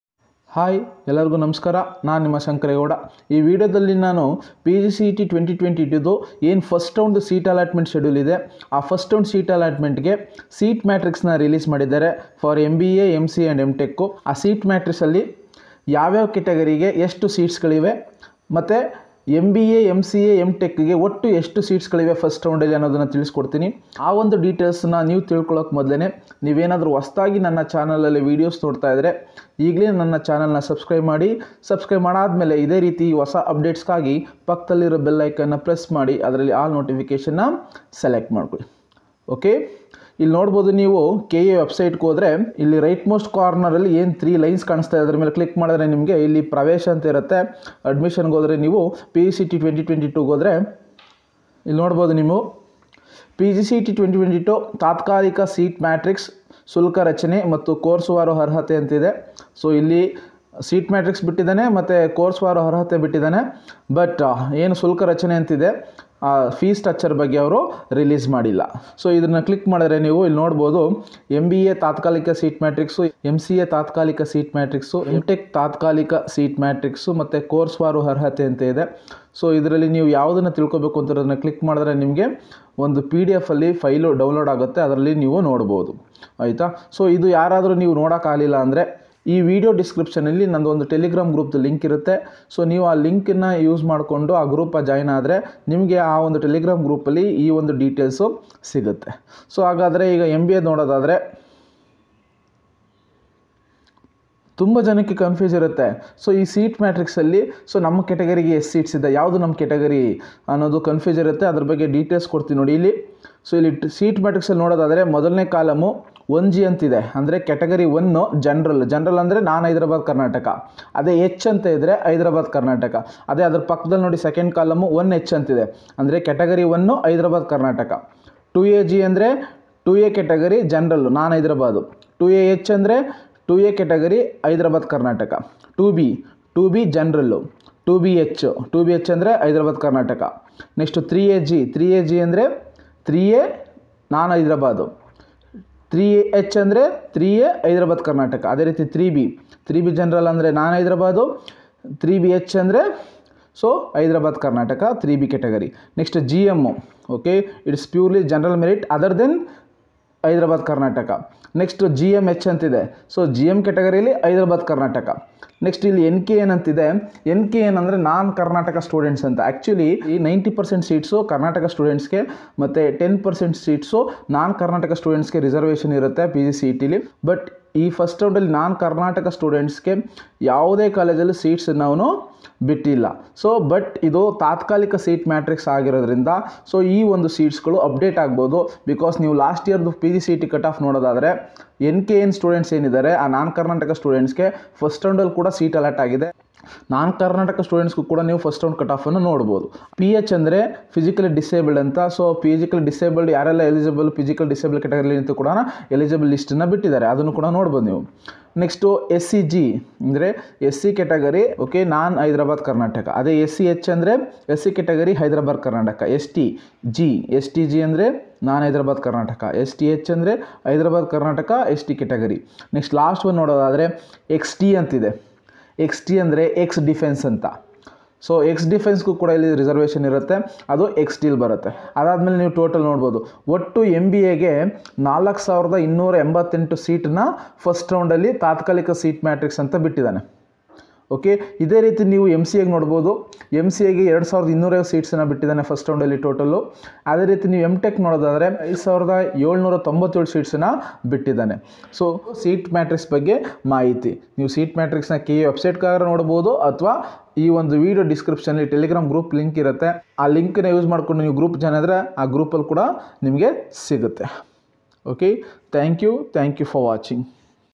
|PGCET-202|| First Round Seat Matrix for MBA, MCA and M.Tech|Totally How many Seats?||Category Wise|
For Immediate updates join my telegram group.

This video about:
In this video I explained about first round seat matrix for MBA, MCA and M.Tech.